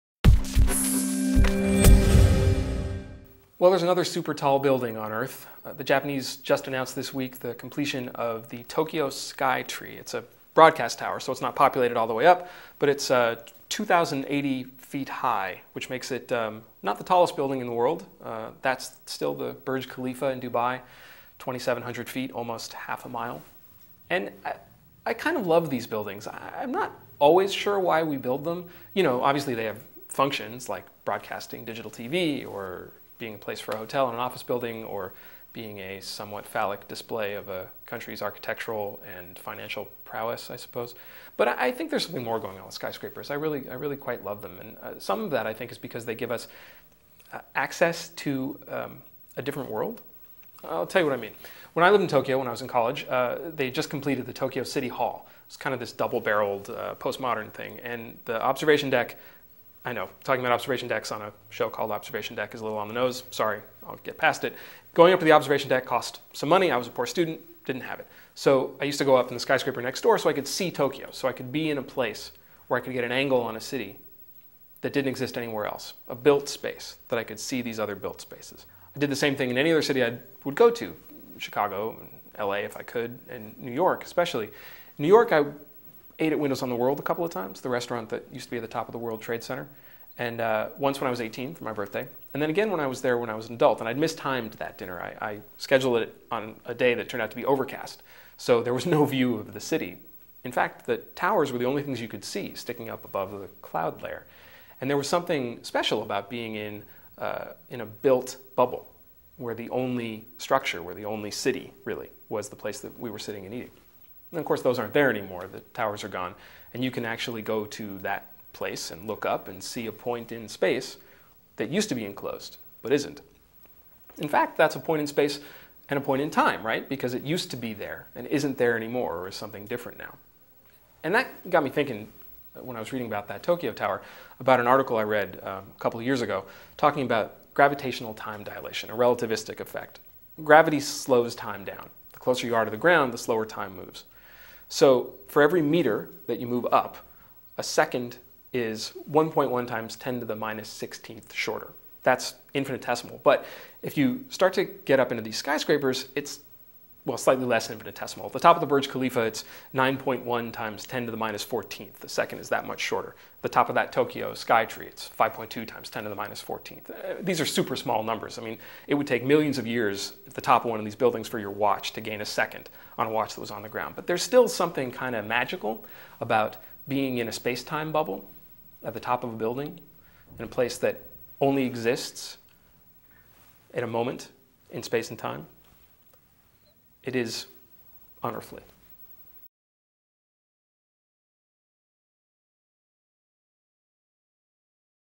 Observation Deck: The Time-Warping Power of Skyscrapers
Wired Senior Editor Adam Rogers talks about skyscrapers and relativity in this week's Observation Deck.